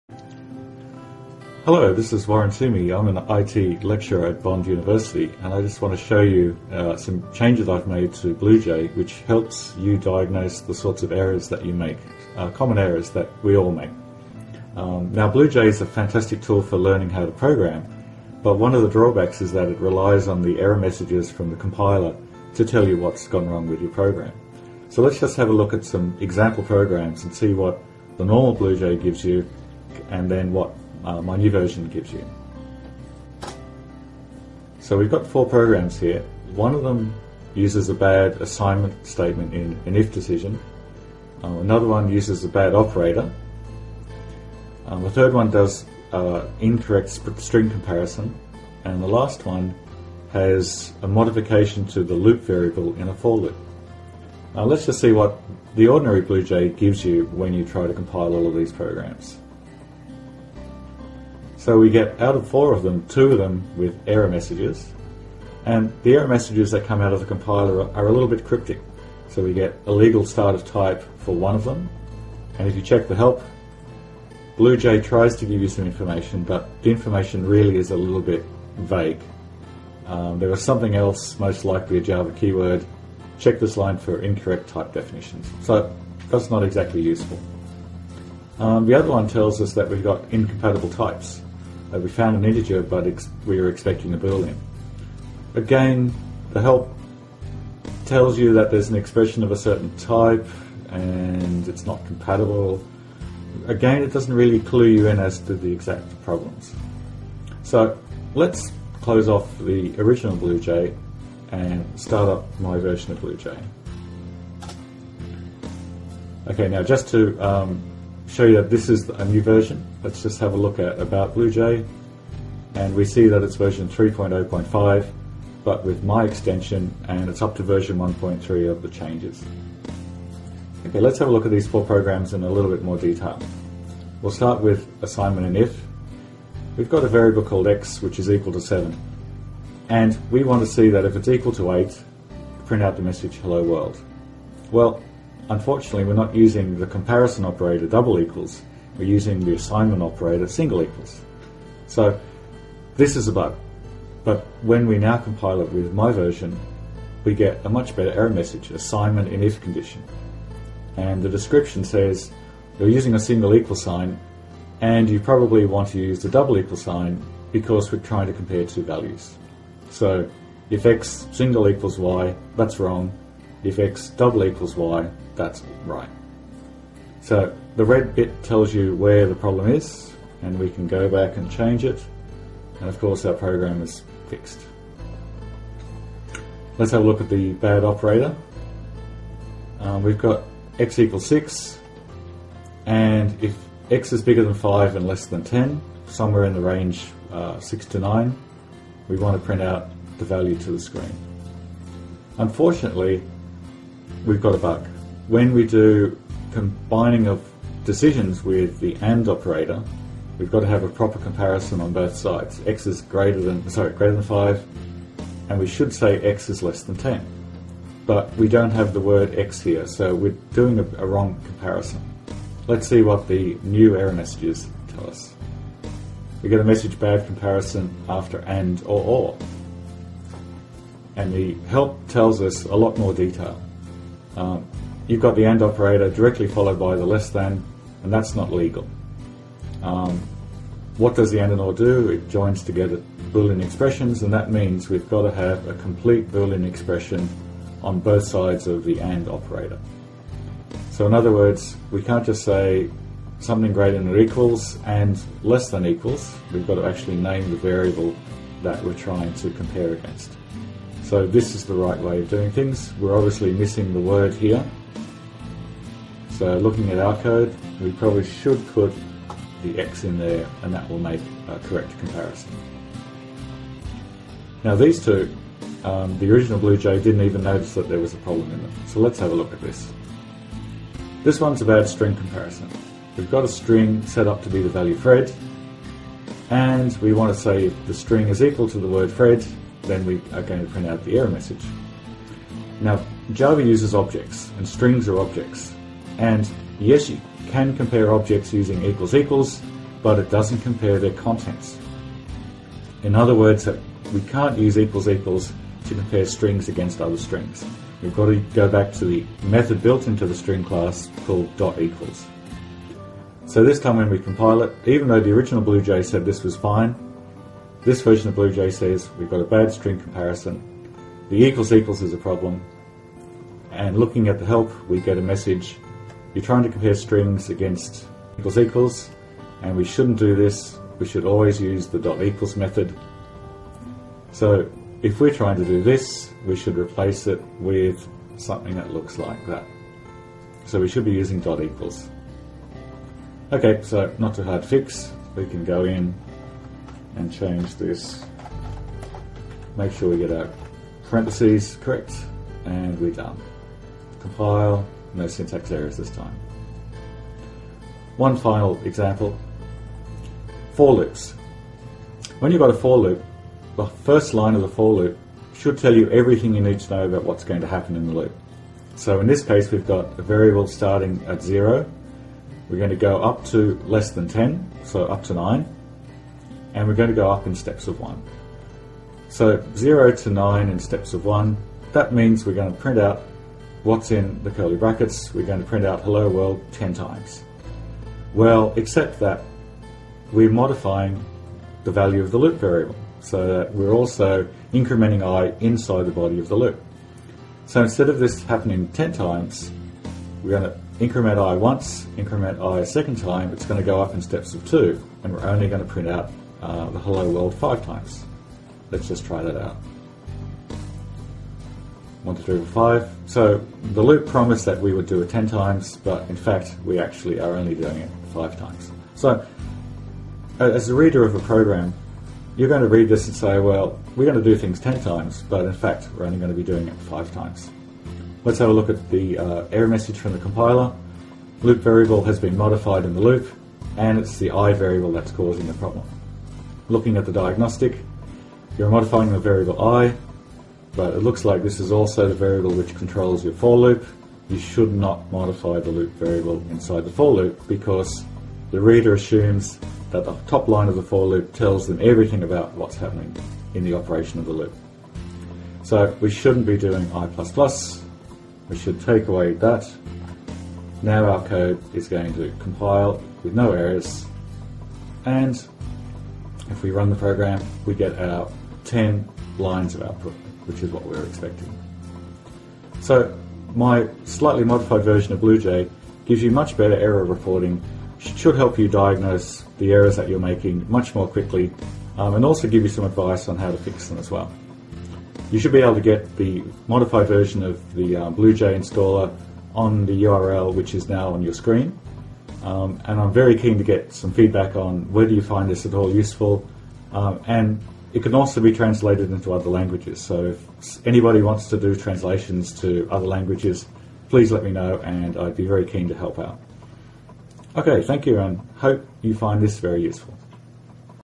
Better Error Reporting with BlueJ
Novice programmers often make simple errors, but they don't receive very good advice on how to fix the errors from the development tools such as the compiler: obscure reports such as "char cannot be dereferenced", for example, give no useful guidance to the student. This is a modified version of BlueJ which has had the error reporting subsystem rewritten to find common programming errors that novice programmers make, report these errors back in a descriptive way to reiterate the core concepts of programming, and give advice that the student can follow to fix the problem.